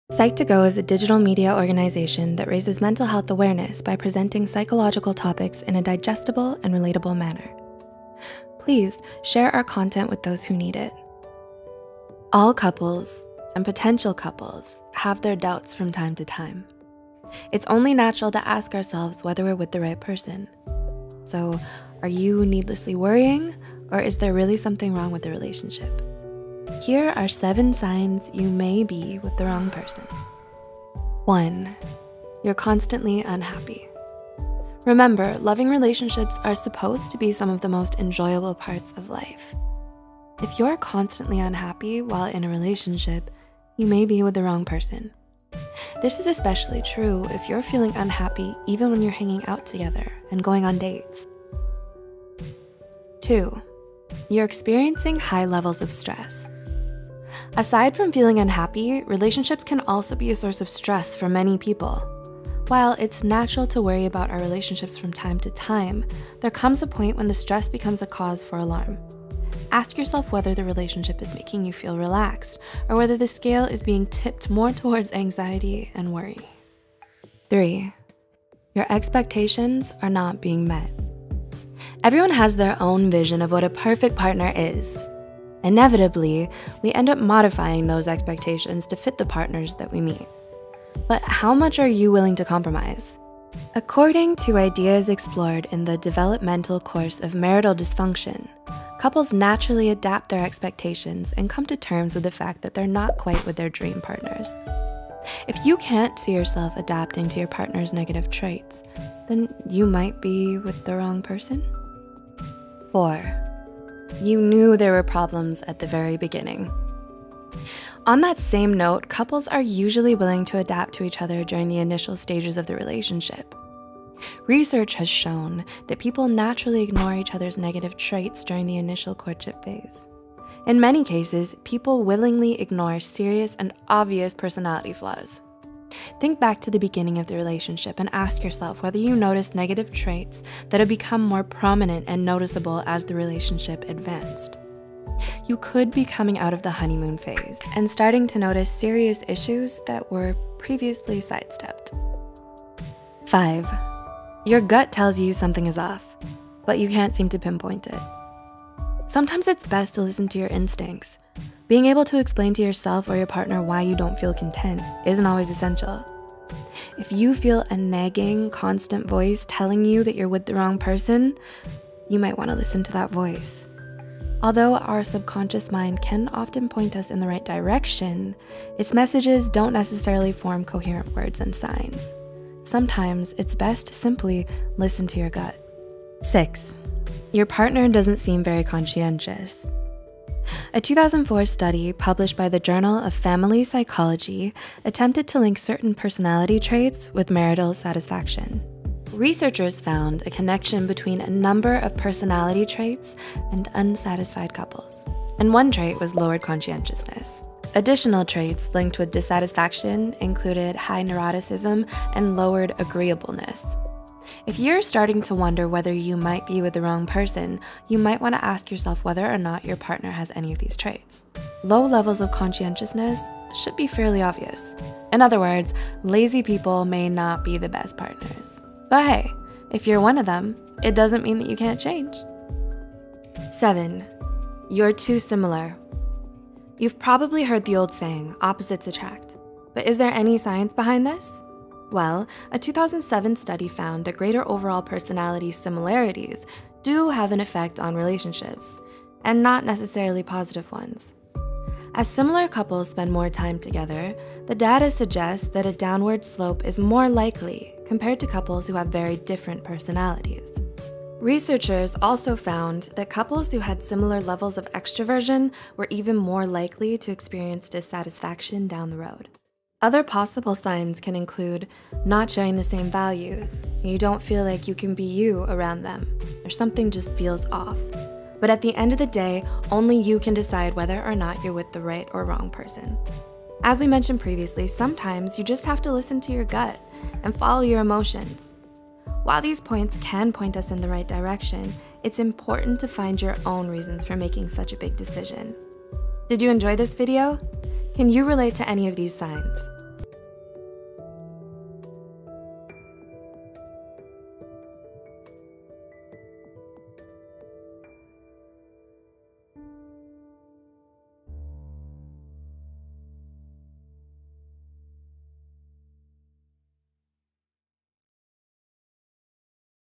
7 Signs You May Be With The Wrong Person
Are you currently in a relationship or dating someone? Do you wonder if your boyfriend or girlfriend is the right person for you? Does your gut feelings or emotions tell you that something is wrong with the relationship? If you're looking for some relationship advice, then this video is for you. All couples or potential couples have their doubts from time to time.

Credits
Script Writer: Elliot Figueira
Script Editor: Kelly Soong
VO: Amanda Silvera
Animator: Grace Cárdenas Cano
YouTube Manager: Cindy Cheong

References:
You Can't Seem To Modify Your Expectations To Fit Your Partner

You Knew There Were Problems When You First Started Dating, But You Suppressed Those Feelings
- same link as above; the passage basically states that in the initial courtship stages, people often willingly ignore obvious problems. They're seeing everything through "rose-tinted glasses."

You're Too Similar
This study found that greater personality similarity is linked with lower marriage satisfaction